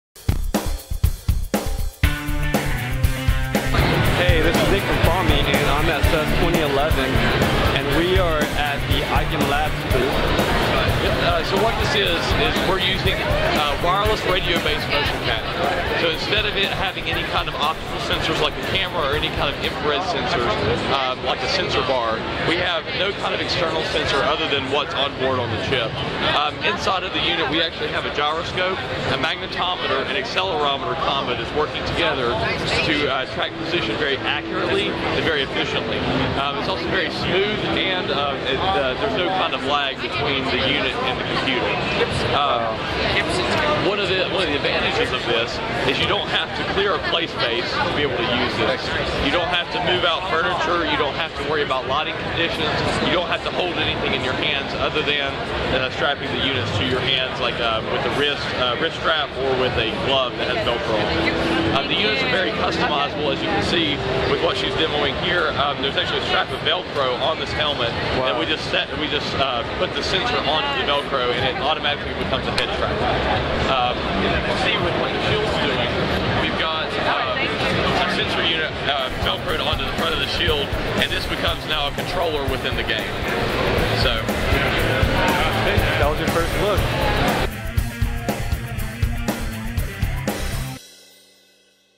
Aiken Labs Booth at CES 2011 in HD Quick look at Future of Gaming
Game systems like the Wii, Playstation Move and Microsoft Xbox Kinect have exploded on the gaming scene, but one drawback is that the only way you can control the game is with special controllers.

Now Aiken Labs has come up with a Modular motion sensing game kit that allows gamers to stick their devices on anything they can think of and put those objects into the game. Aiken has put out tiny boxes that come with a full inertial measurement package that routs all their readings back to a base unit that also acts as a charging unit for the different devices.